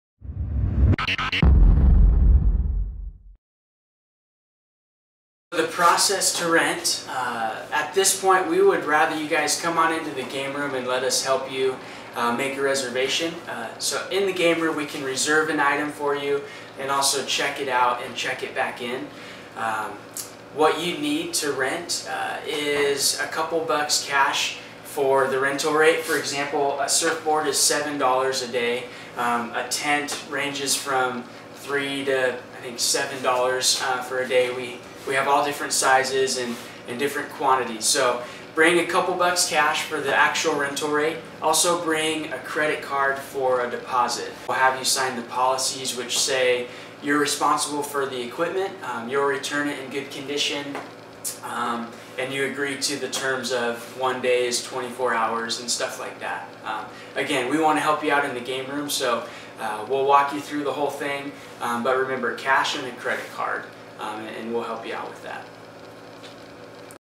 RecRentals
Jon McWhorter explains the rental process for REC RENTALS through California Baptist Univeristy's Community life